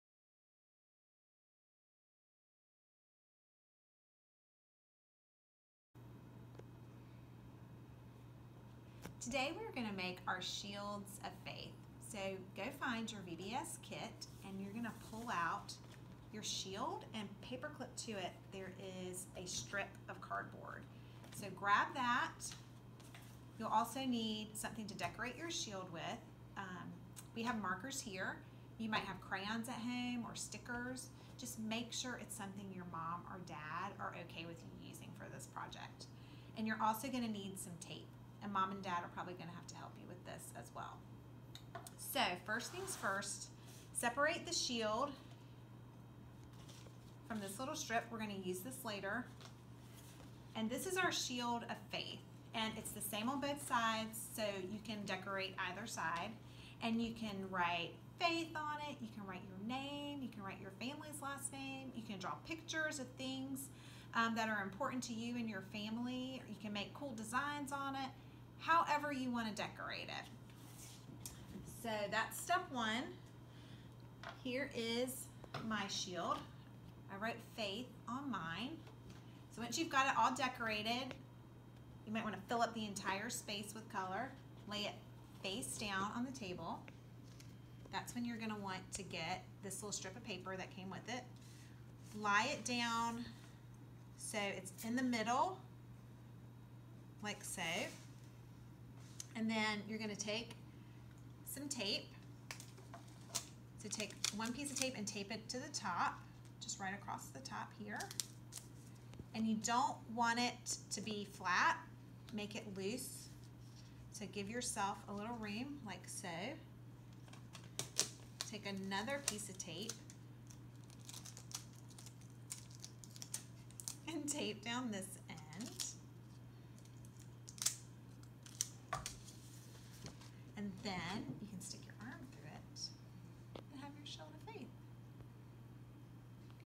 Preschool Art - Shield of Faith (Thursday)
Watch this video to see a demonstration of the Shield of Faith craft.